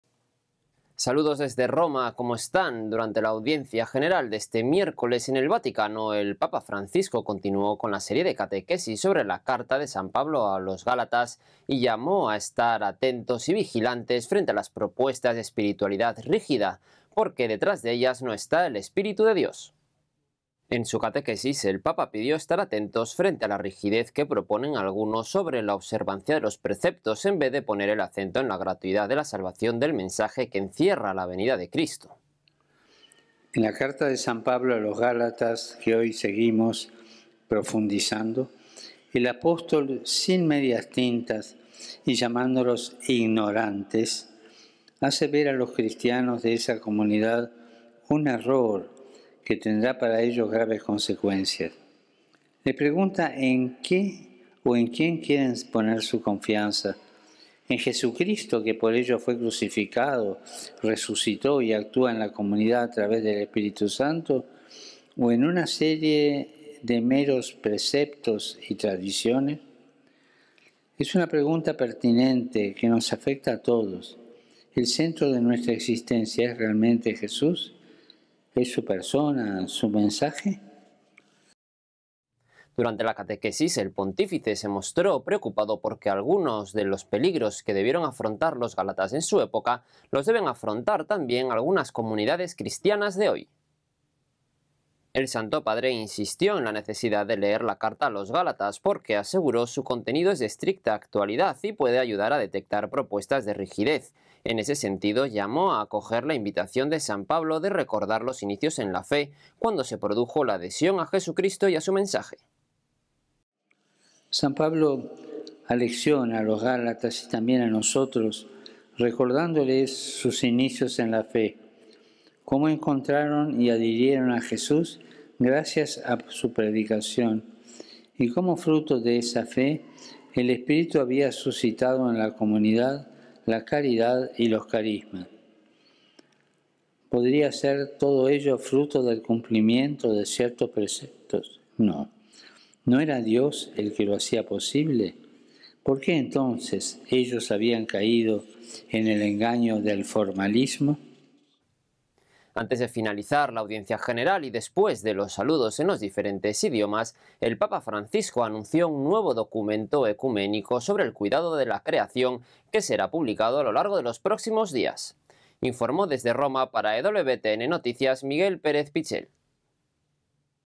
El Papa pide estar vigilantes ante espiritualidad rígida
Nuestro corresponsal de Roma Miguel Pérez Pichel nos trajo información sobre la catequesis que dio el Papa Francisco en su Audiencia General del miércoles y en la que llamó a estar vigilantes frente a las propuestas de espiritualidad rígida.